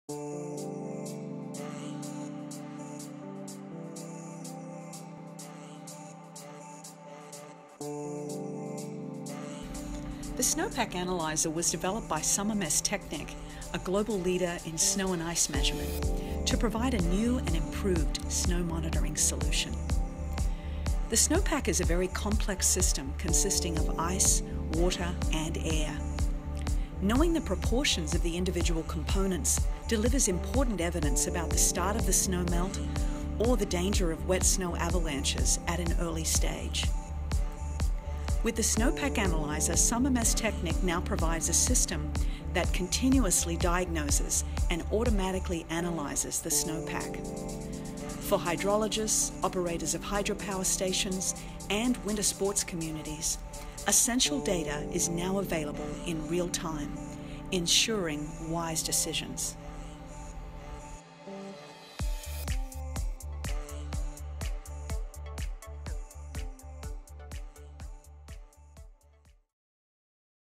Snow Pack Analyser (SPA) System by SOMMER Messtechnik
The SnowPackAnalyser measures the volumetric contents of ice, water and air together with the snow depth and calculates the snow-water-equivalent (SWE) and the snow density.

The SPA allows predictions about the snow melting process and the expected run-off water quantity, through continuous and automatic measuring of all relevant parameters of the snow pack.